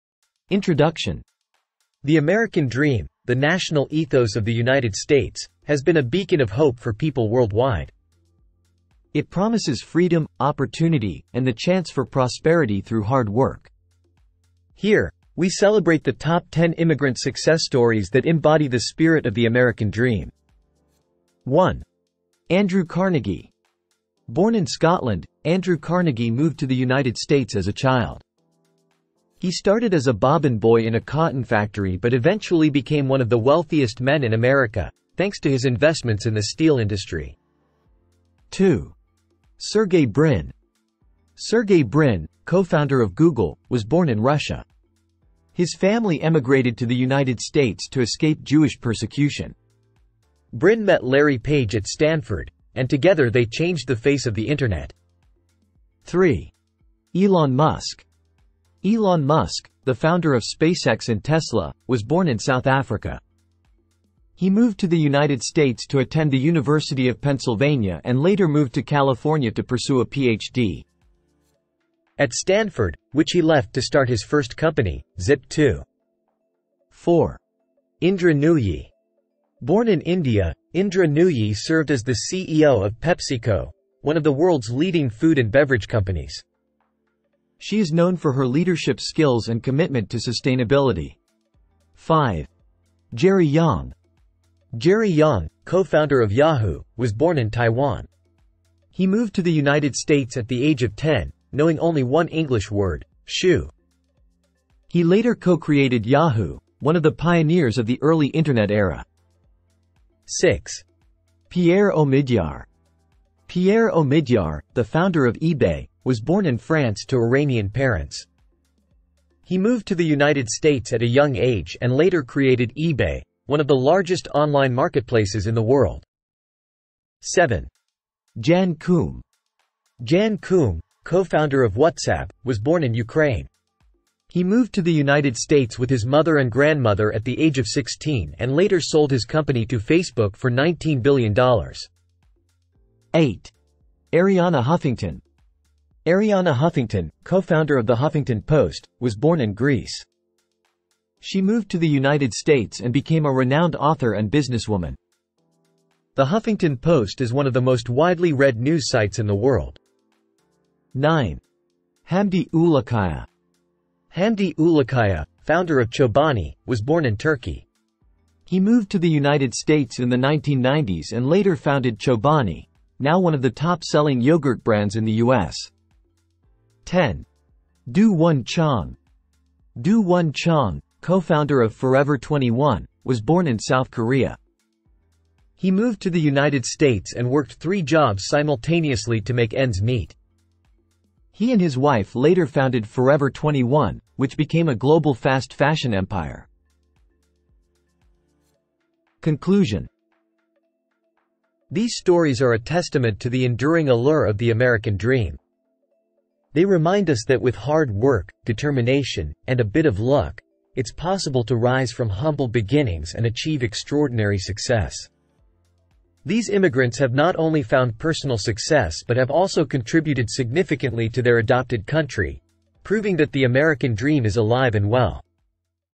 Top 10 Immigrant Success Stories Living the American Dream
Celebrate the achievements of immigrants who have realized the American Dream with our top 10 success stories. From humble beginnings to extraordinary success, these stories highlight the contributions and accomplishments of immigrants in America.